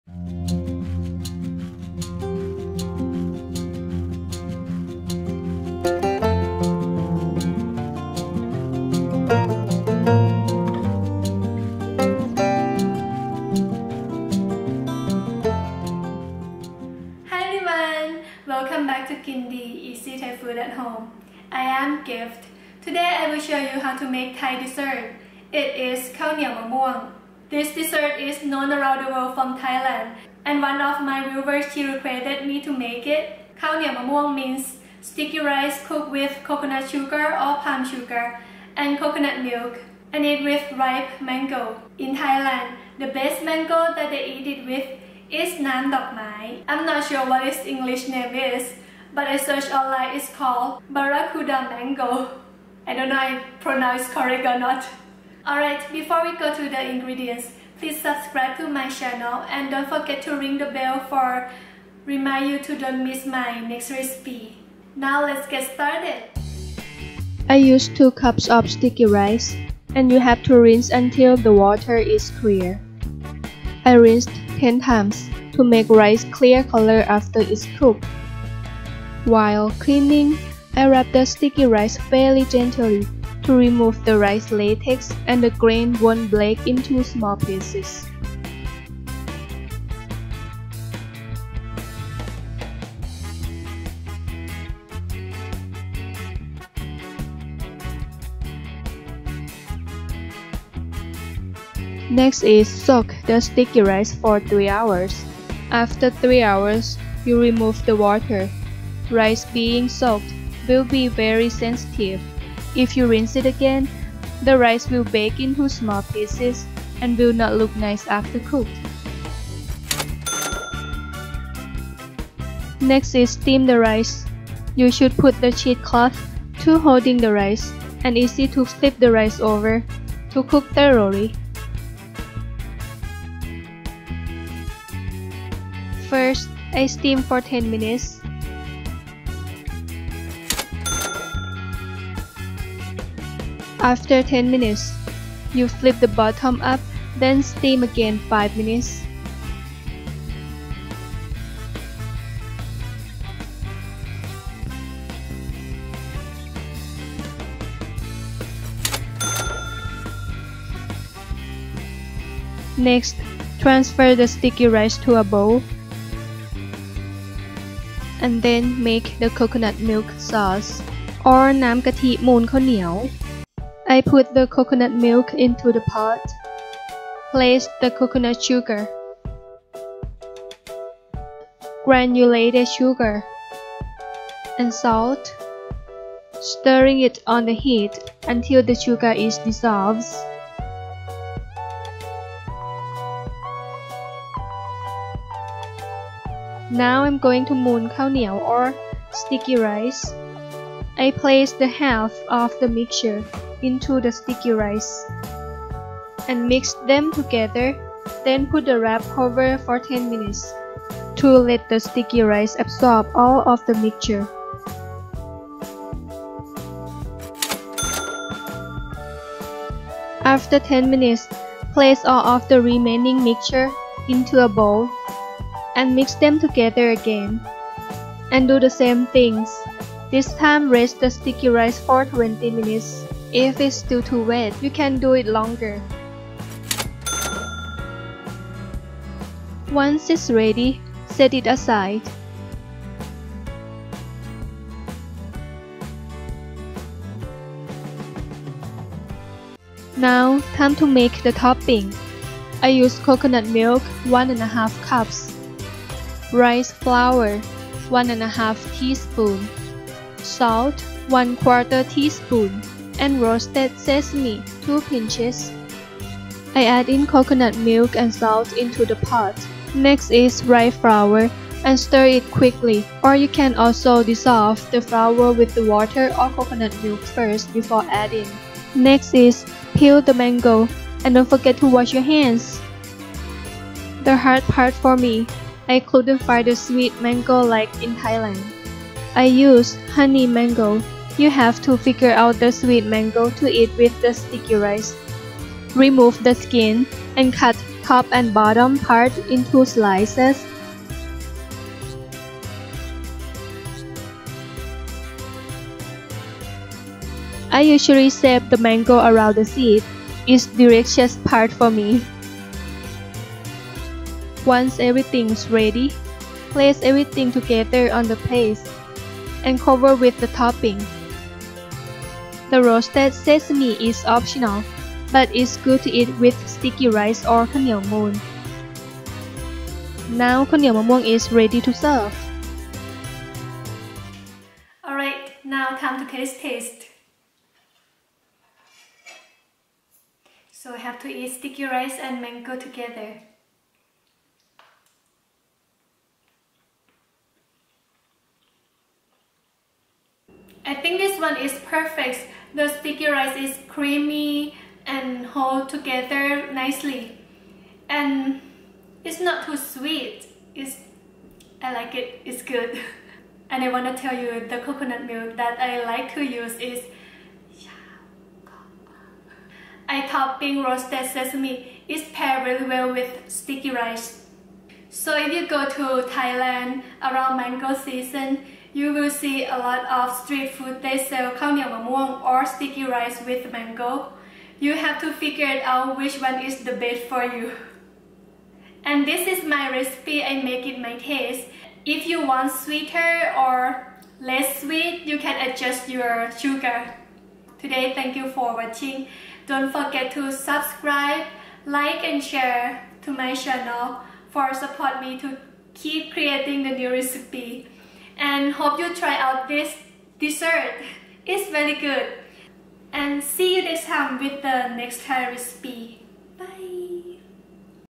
Creamy sticky rice with ripe mango recipe KAW NEAW MA MUANG ข้าวเหนียวมะม่วง
This sticky rice is nicely sweet and creamy. Also you have to eat with sweet mango. And topping with coconut milk sauce it very good. I hope you try out this dessert.
Ingredients

Sticky rice 2 cups
Mango 2

Nam moon kaw neaw or coconut milk sauce
1. Coconut milk 1 cup
2. Coconut sugar 1/4 cup
3. Granulated sugar 1/3 cup
4. Salt 1/2 tsp
You can drop the food color if you prefer

Coconut milk topping sauce
1. Coconut milk 1/2 cup
2. Rice flour 1 1/2 tsp
3. Salt 1/4 tsp
4. Roasted sesame 2 pinches